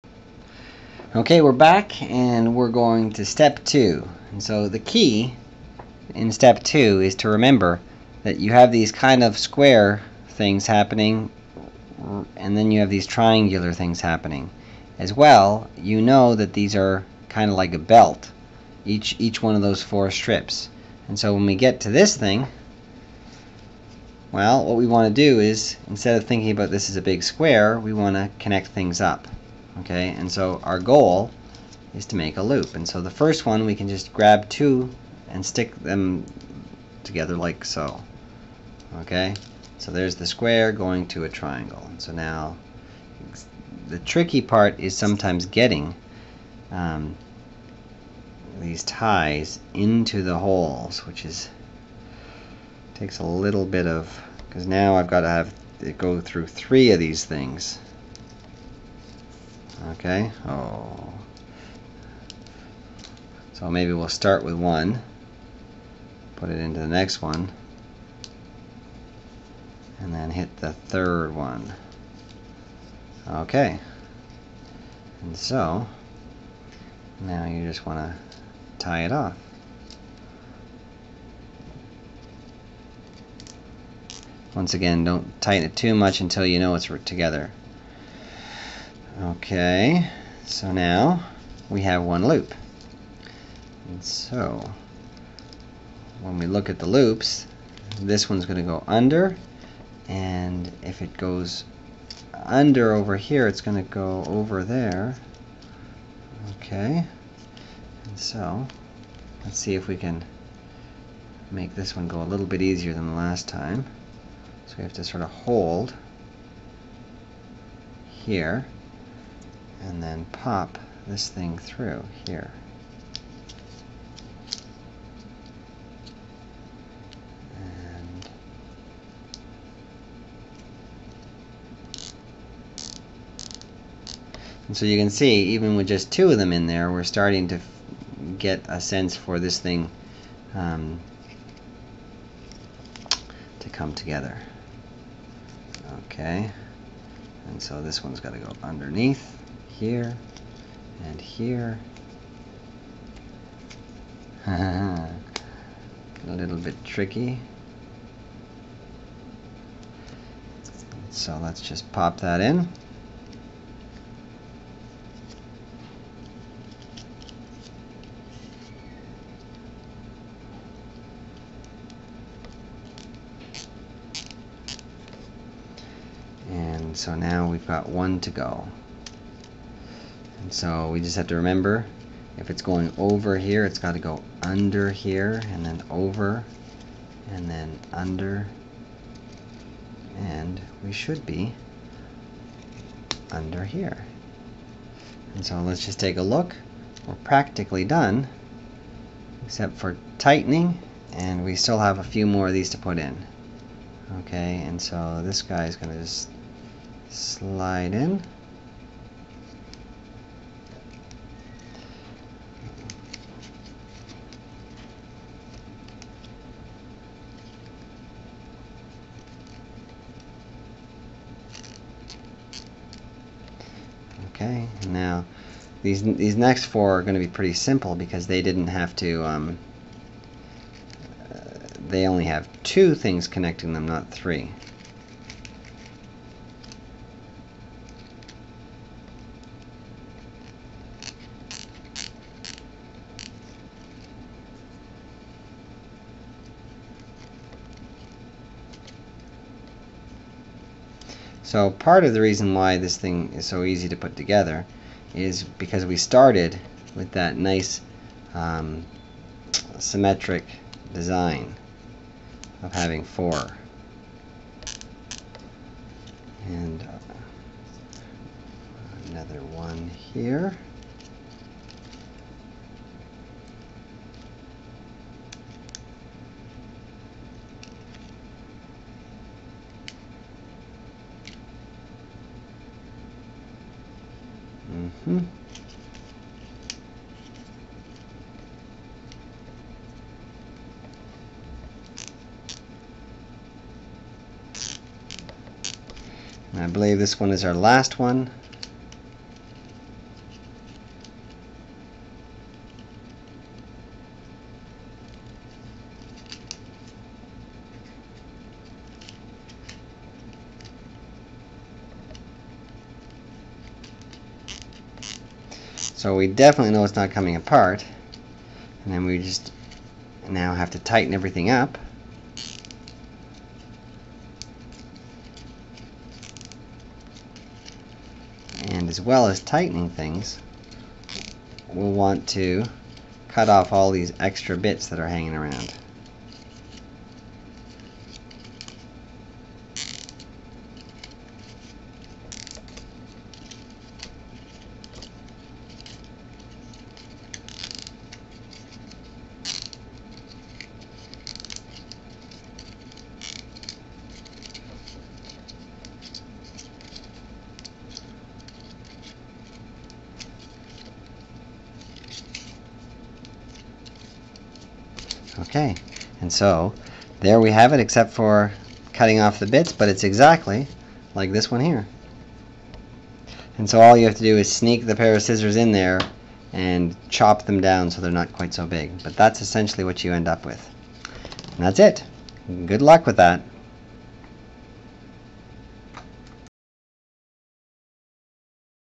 Making more polyhedra (step 2)
This is the second part for making a cuboctahedron out of hanger tape and cable ties